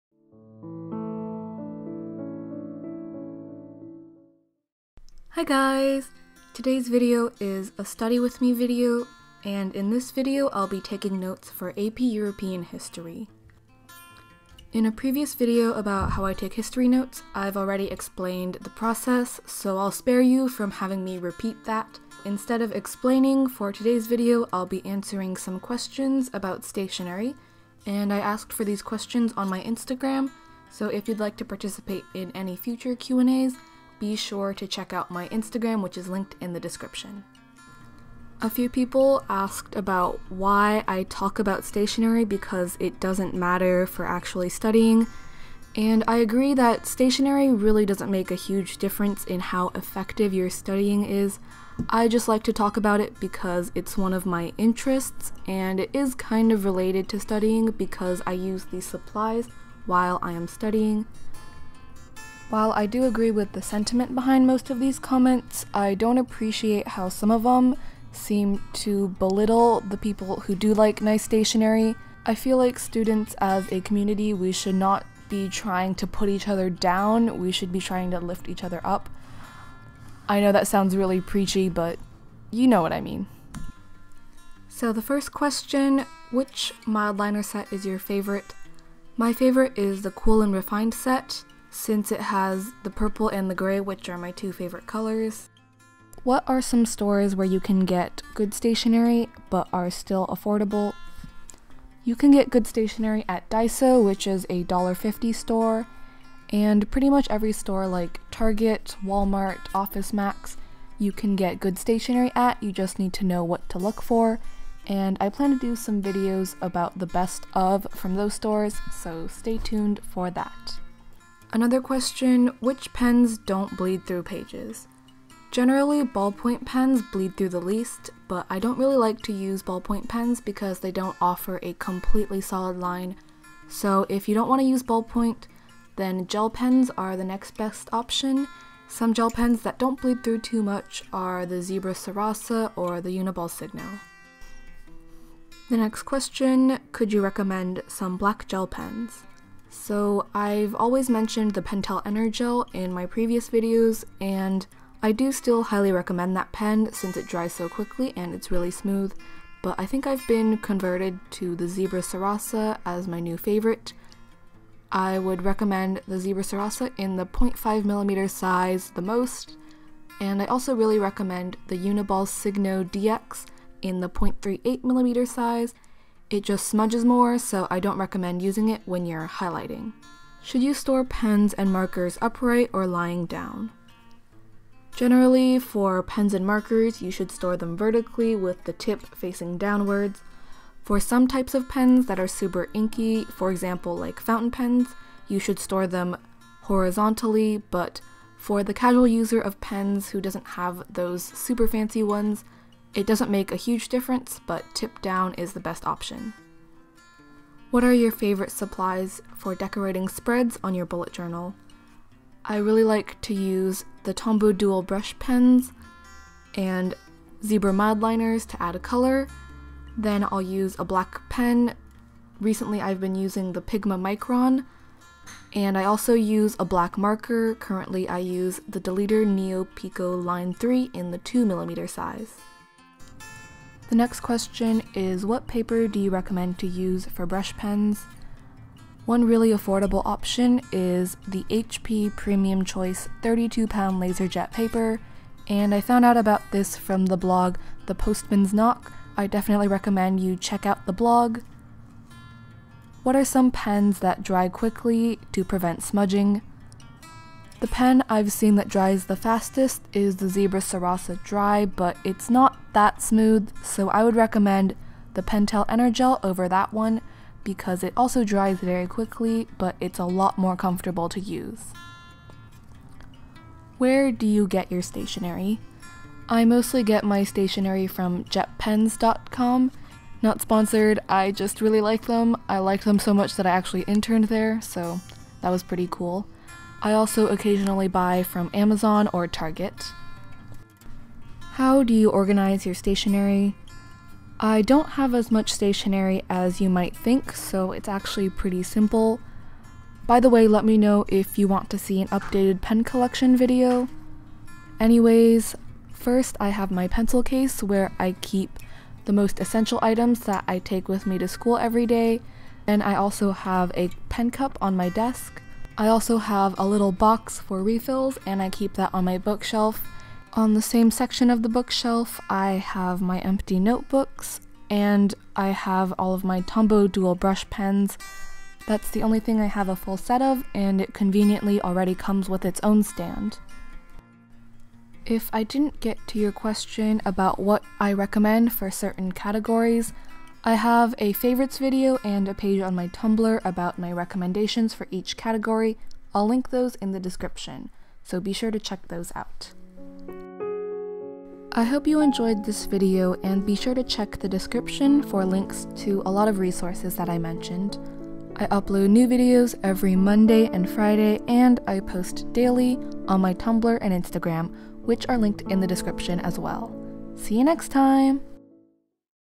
how i study for ap history 🌍 study with me
Question: do you prefer study with me videos about one subject or a compilation of one day/week?

Supplies used in video
⋅ OfficeMax binder paper

⋅ HP Premium Choice Laserjet 32# paper

Please comment and leave a rating so I get that sweet validation :')